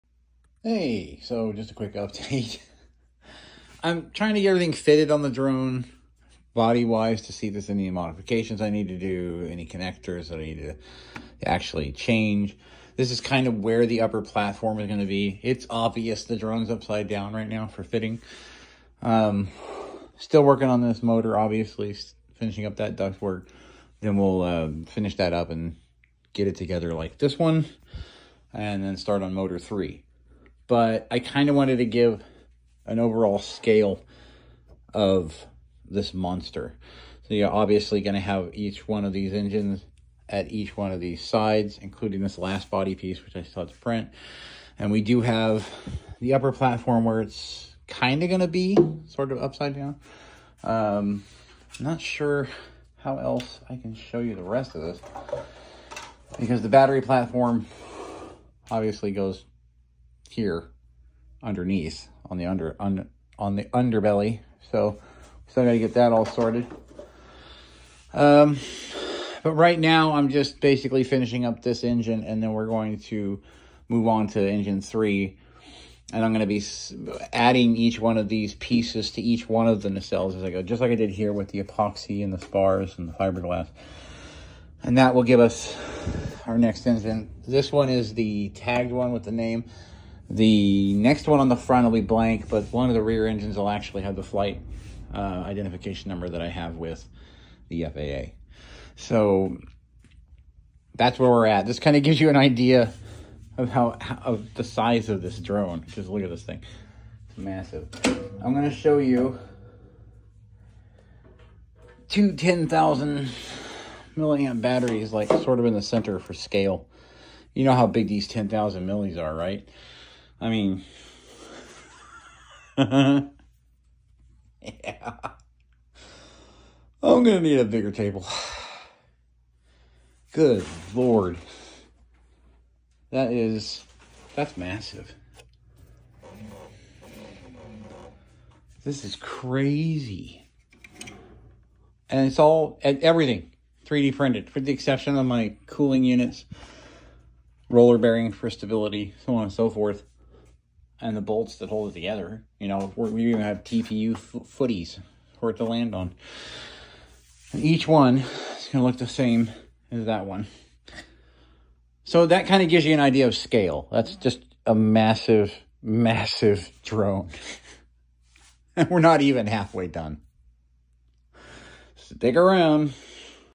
Massive drone assembly the scale of this thing is terrifying. It’s insane!
It does really kind of put this drone into perspective to see the body sections coming together.